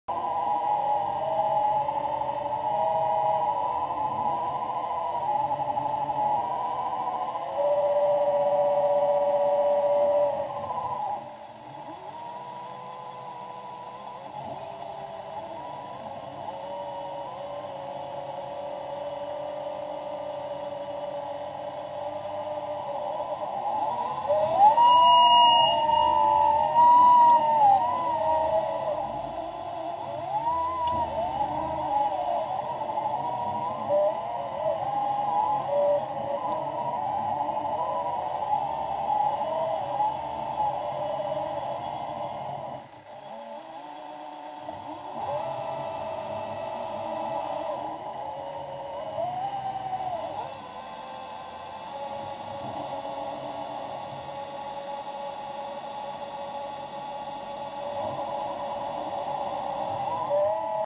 High Definition ROV Video
This short clip demonstrates the quality of the video image recorded on the HD camera aboard the ROV during the Niku VII expedition. We were not able to view this imagery until after the expedition.  The sounds you hear are the thrusters that control the movement of the vehicle.